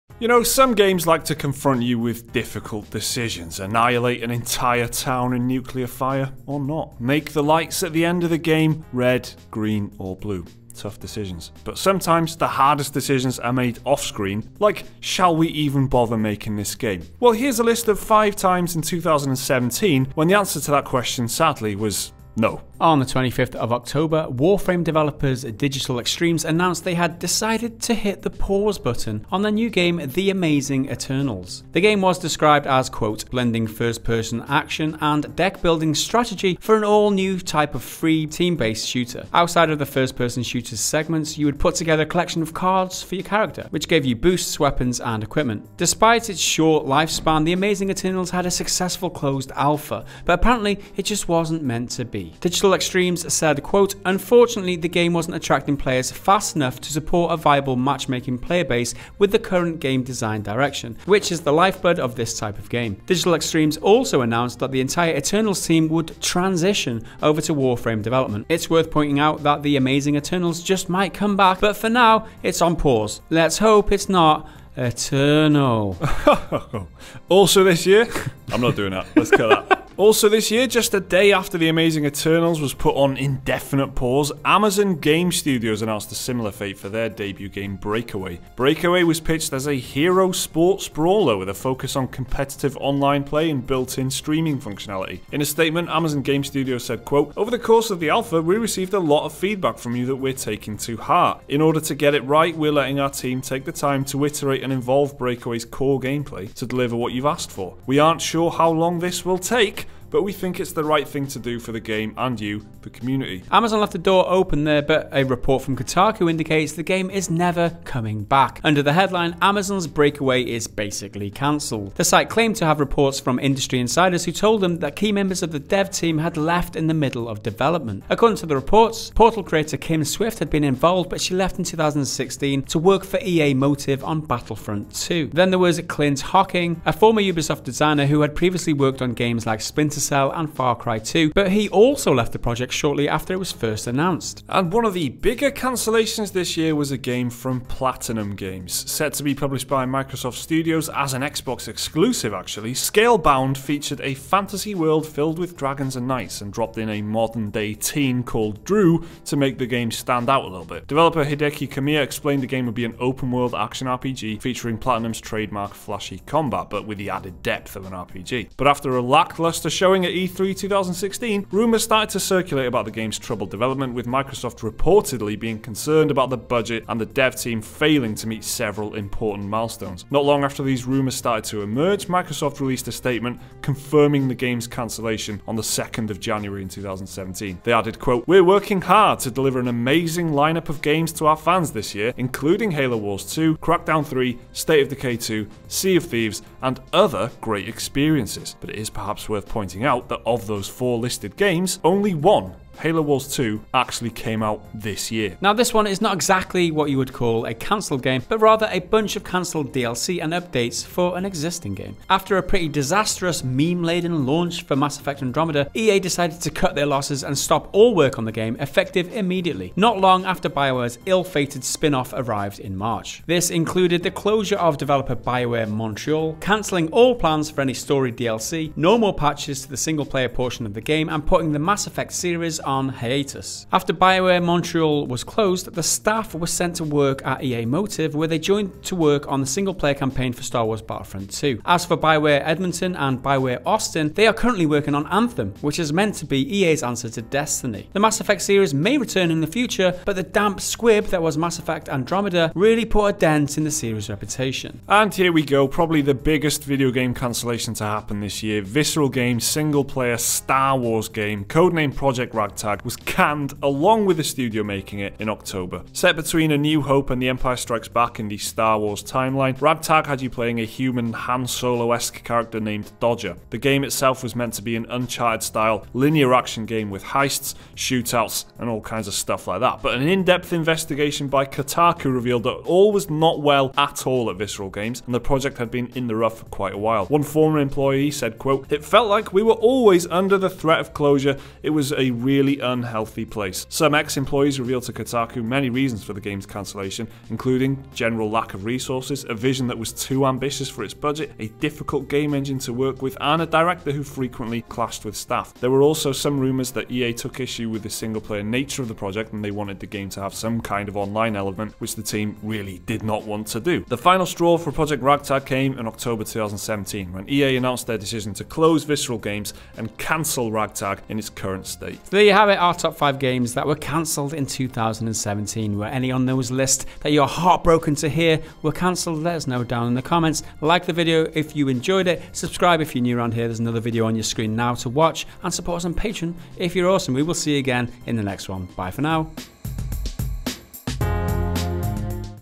Top 5 Games Cancelled in 2017
We may have seen some wonderful games released this year and exciting projects announced for the future. But 2017 also had its fair share of casualties, with several highly anticipated games being either cancelled, put on hiatus or just forgotten about. Here’s a a list of the most disappointing gaming cancellations this year, just incase you’re feeling a bit too cheerful this Christmas.

Hosts: Mike Williams and Gareth Evans
Script: Jake Jones and Lucy Jones
Script Editing: Mike Williams and Liam McKelvey
Editor: Gareth Evans